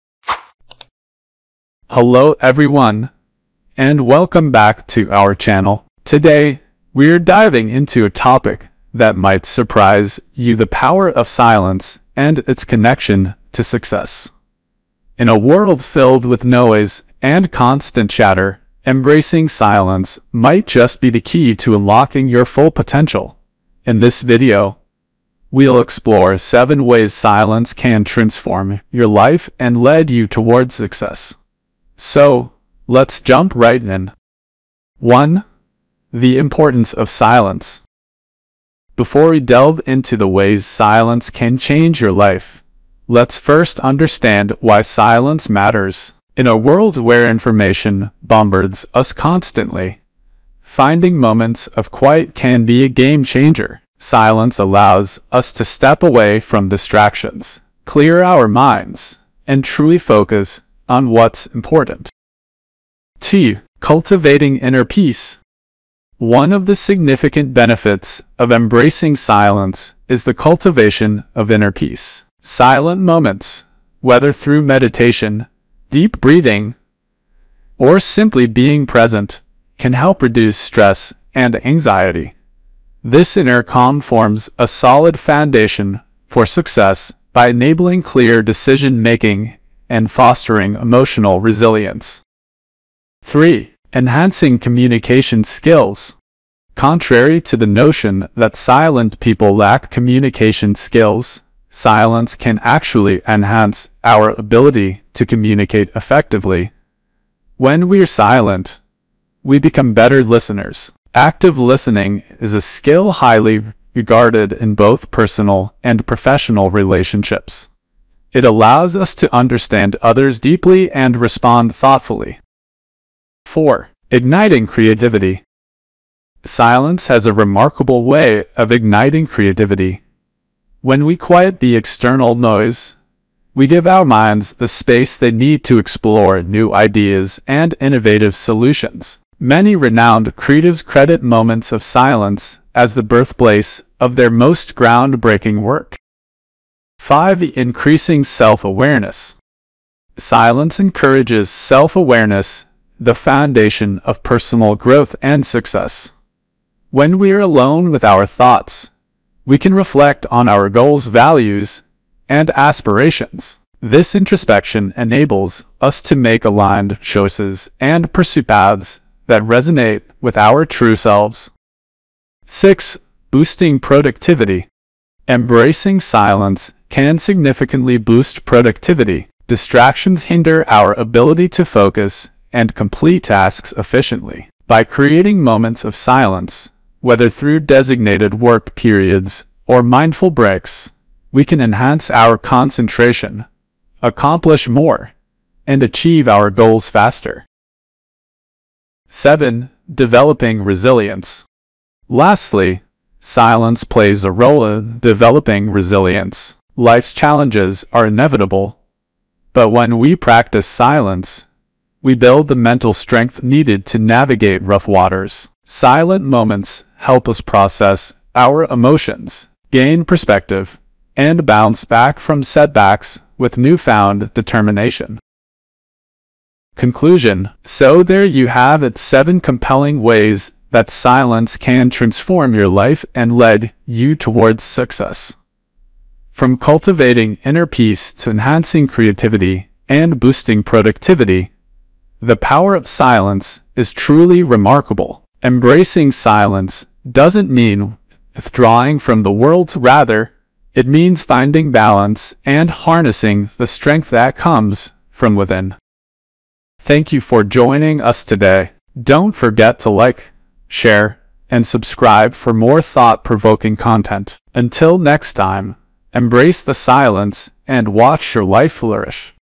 The Power of Silence | Why Silent People Are Successful | 7 Ways Silence Will Change Your Life
In this video, we delve deep into a topic that might challenge your perspective – "The Power of Silence: Why Silent People Are Successful | 7 Ways Silence Will Change Your Life." In a world filled with constant noise and distractions, the idea of embracing silence might seem counterintuitive, but you'll be amazed at how it can reshape your journey to success.
Segment Breakdown:

1. The Importance of Silence: Silence isn't about withdrawal; it's about creating space to truly focus. Discover how embracing silence can transform your perspective and help you see what truly matters.

2. Cultivating Inner Peace: Learn how silence can be a powerful tool for reducing stress, anxiety, and emotional clutter. Inner peace becomes the foundation upon which your success story is built.

3. Enhancing Communication Skills: Contrary to common belief, silence can actually enhance your communication skills. Active listening and thoughtful responses are just some of the benefits.

4. Igniting Creativity: Unleash your creative potential by embracing moments of silence. Discover how silence sparks innovative ideas and fosters artistic breakthroughs.

5. Increasing Self-Awareness: Self-discovery happens in the quiet moments. Explore how silence enables you to connect with your aspirations, values, and personal growth.

6. Boosting Productivity: Silence is your secret weapon for heightened productivity. Say goodbye to distractions and hello to focused work periods and mindful breaks.

7. Developing Resilience: Life is full of challenges, but silence equips you with the mental resilience needed to bounce back stronger than ever.

In this video, we'll show you how silence can be your ally in the pursuit of success. From cultivating inner peace to unlocking creativity and boosting productivity, you'll gain insights that might just change the way you approach life.

00:01 Introduction
00:35 The Importance of Silence
1:02 Cultivating Inner Peace
1:29 Enhancing Communication Skills
2:04 Igniting Creativity
2:27 Increasing Self-Awareness
2:52 Boosting Productivity
3:18 Developing Resilience
3:47 Conclusion